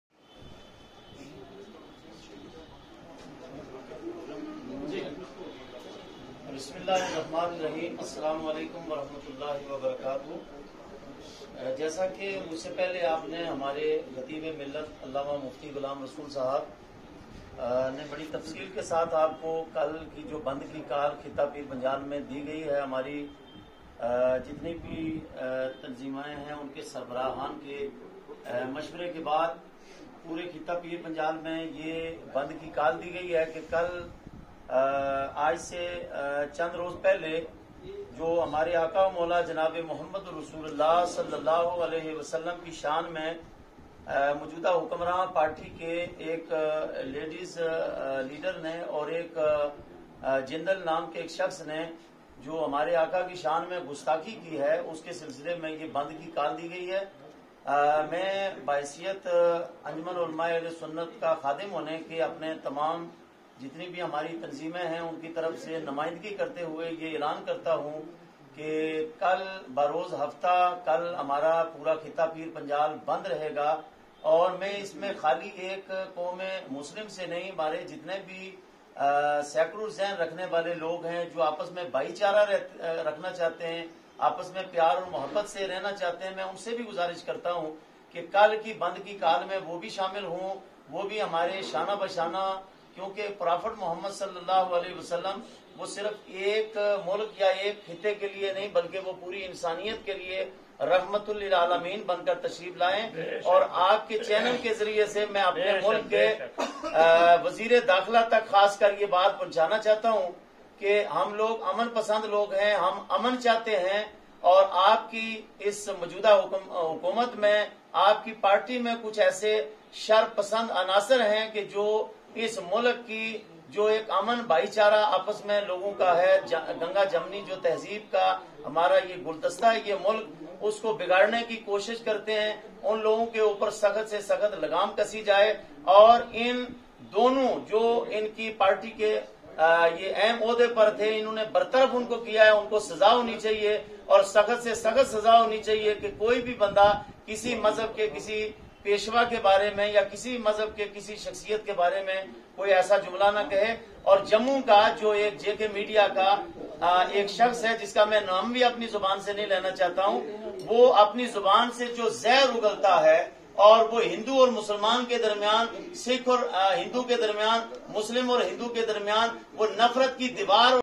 peaceful Bandh call for Saturday by Muslim organizations in Rajouri Poonch distt.
Islamic Welfare Organization and the Ulema Organization called for the Shutdown of Rajouri Poonch on Saturday. Anjuman-e-Tajraan Rajouri announced its support for the shutdown call.